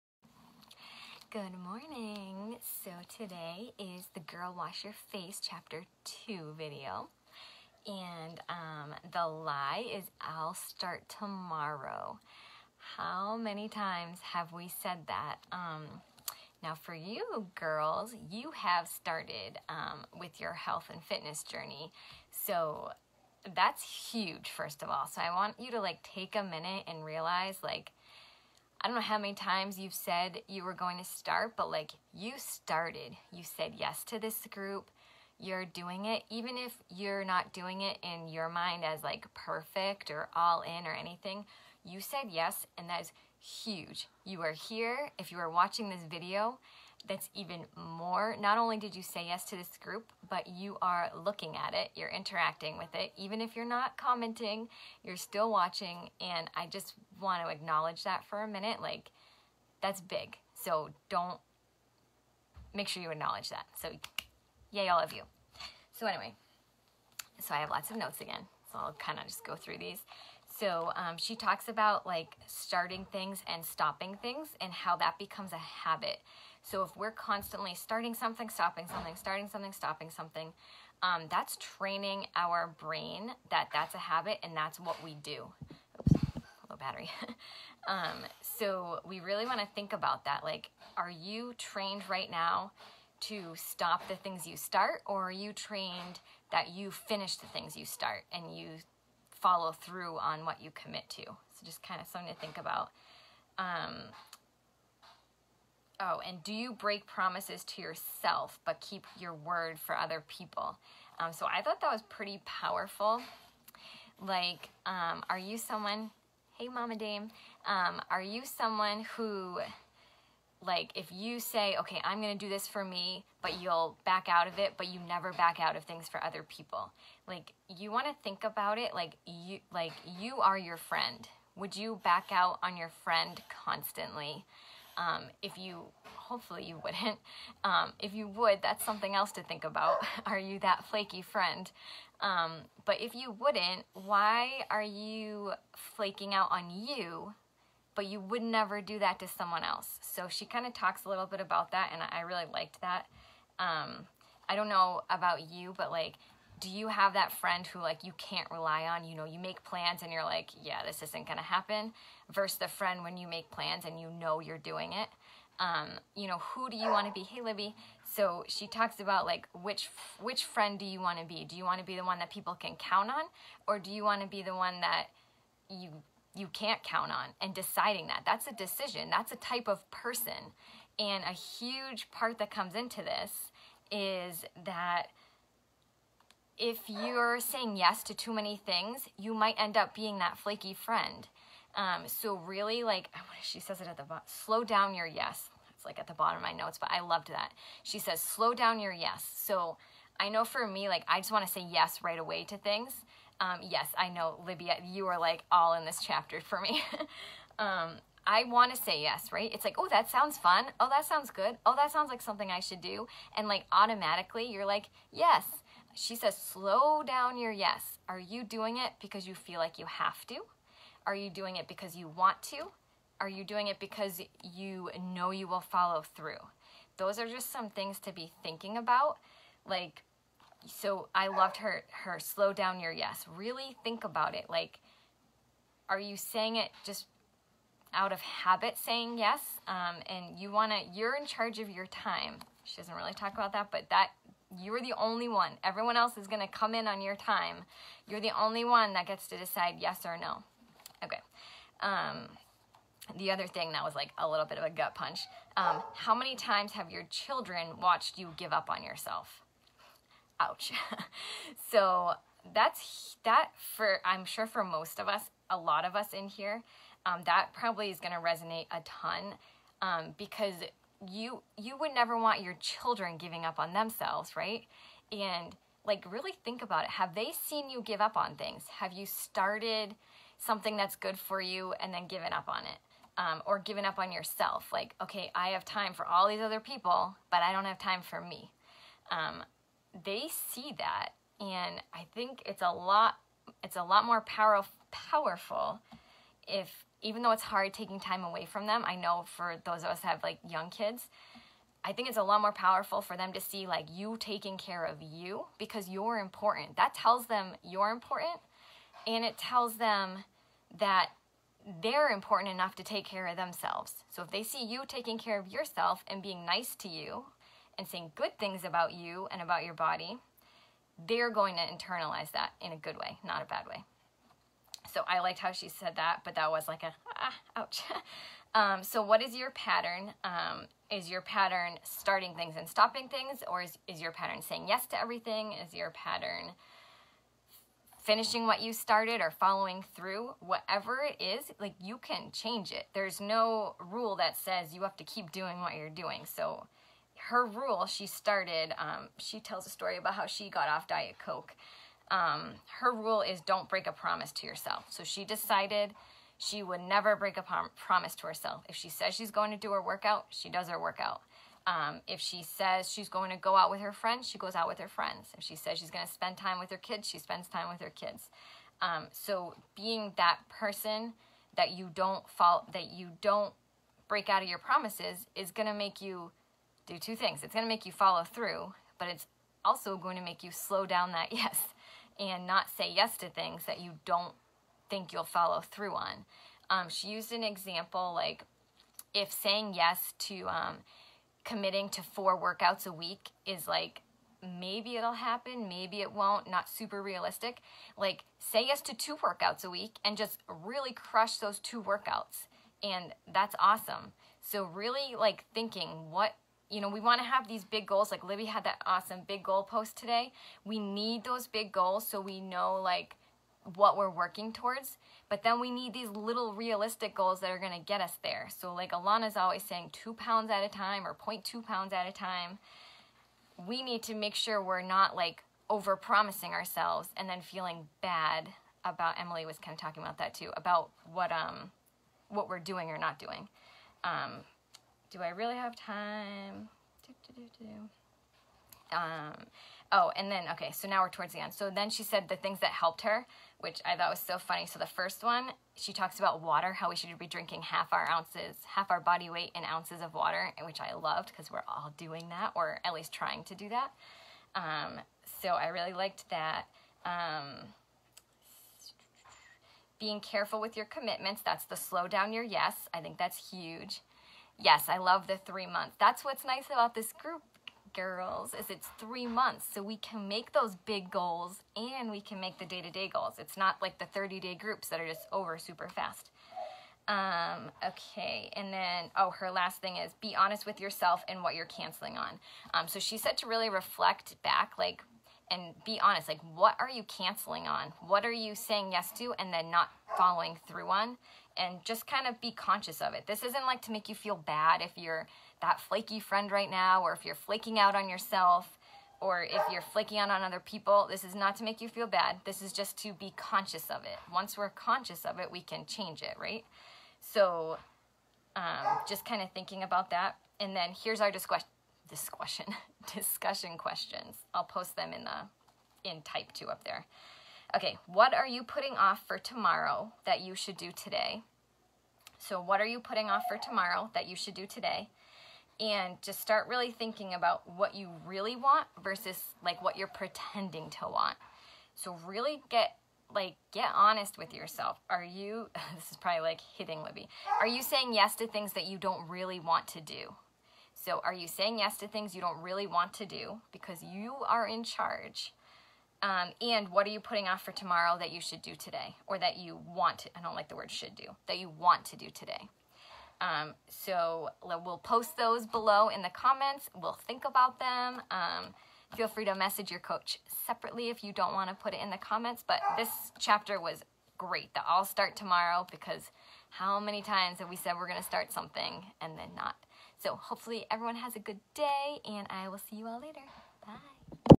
Fall in love with you - Chapter 2 of Girl wash your face
Girl wash your face Chapter 2
The lie: I'll start tomorrow

Discussion Question

What you are putting off till tomorrow that you are ready to start today?

Also think about it: What do you really want versus what are you pretending to want